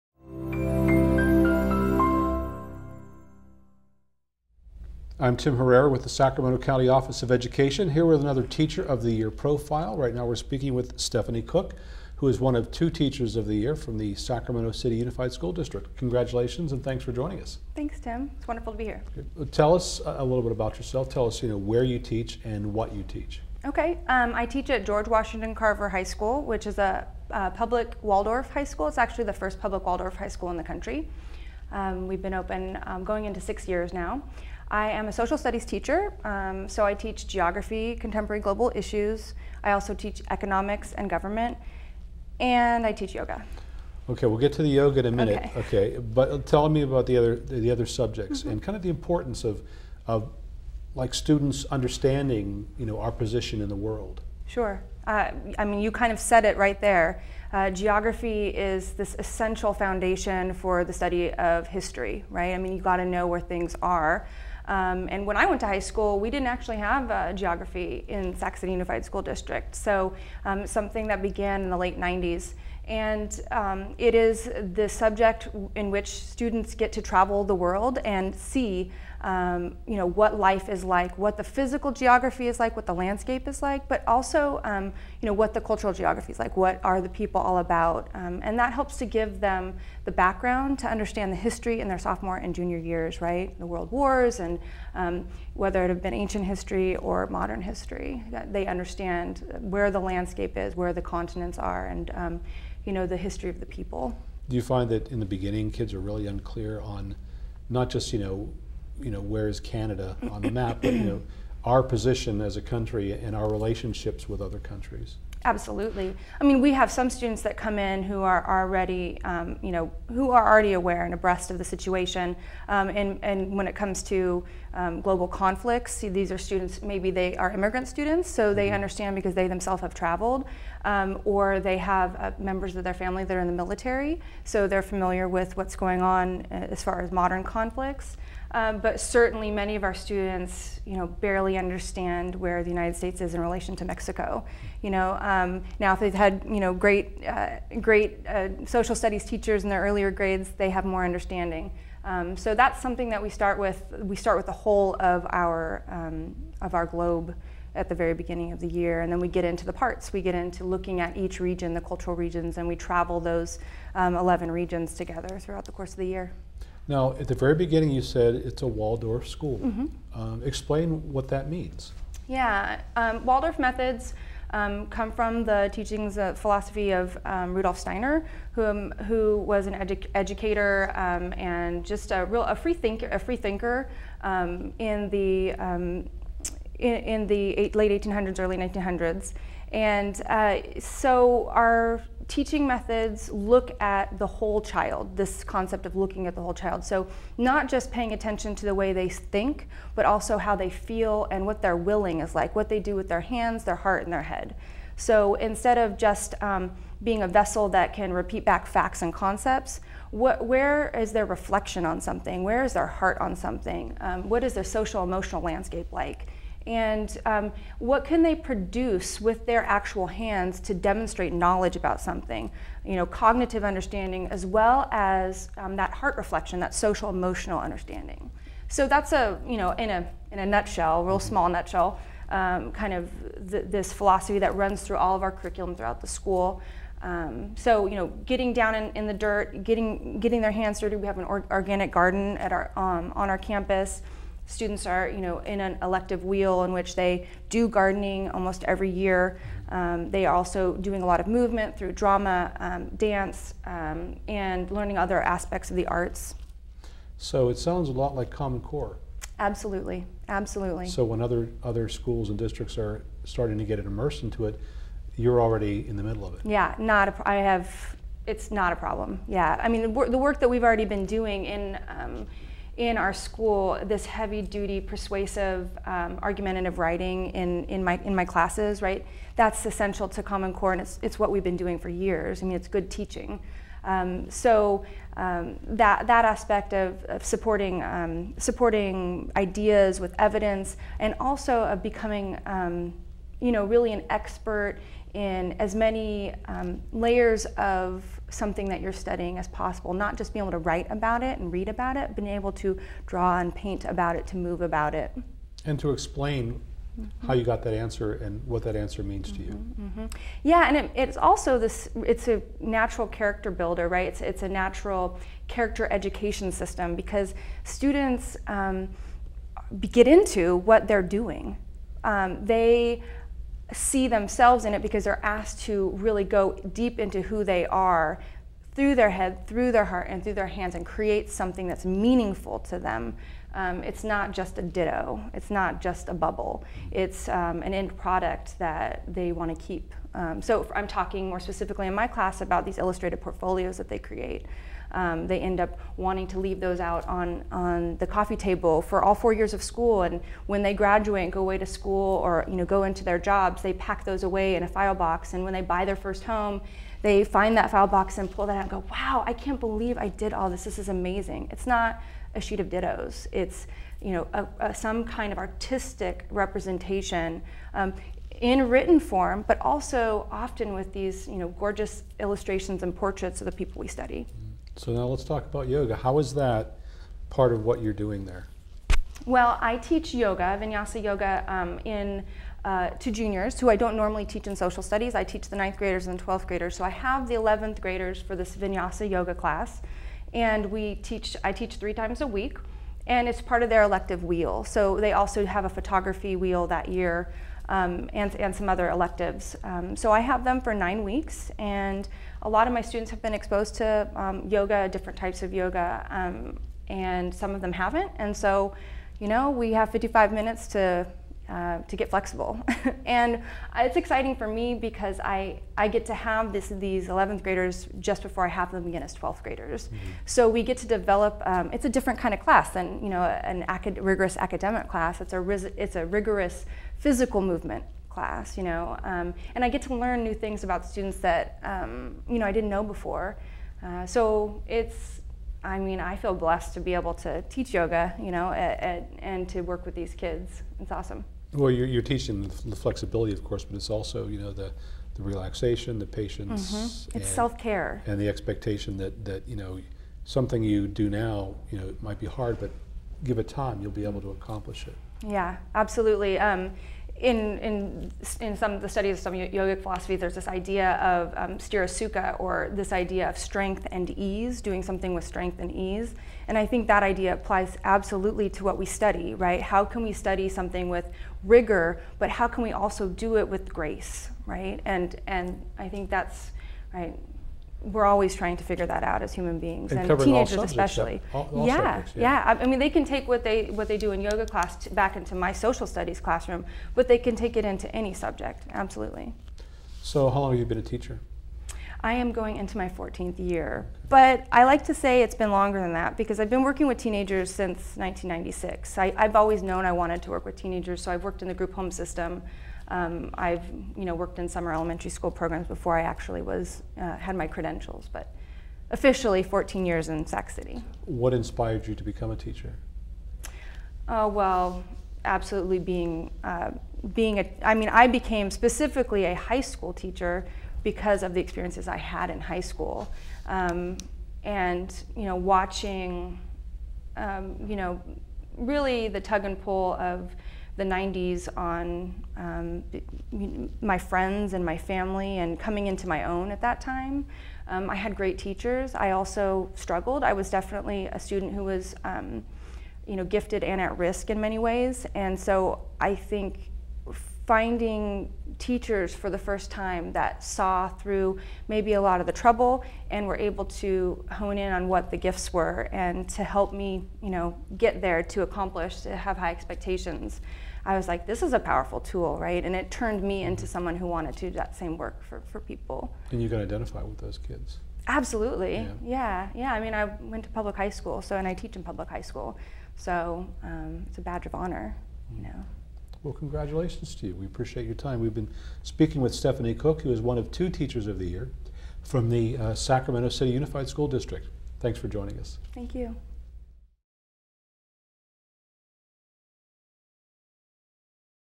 Stephanie Cook: Sacramento County Teachers of the Year 2014 Interview
An interview with one of two Sacramento City Unified School District Teachers of the Year 2014. The Sacramento County Teachers of the Year program recognizes excellence in teaching within the county's schools. Participating districts nominate individual teachers to receive this special recognition. At the Teachers of the Year awards recognition dinner, two teachers are identified as the Sacramento County Teachers of the Year and are then eligible to participate in the statewide competition.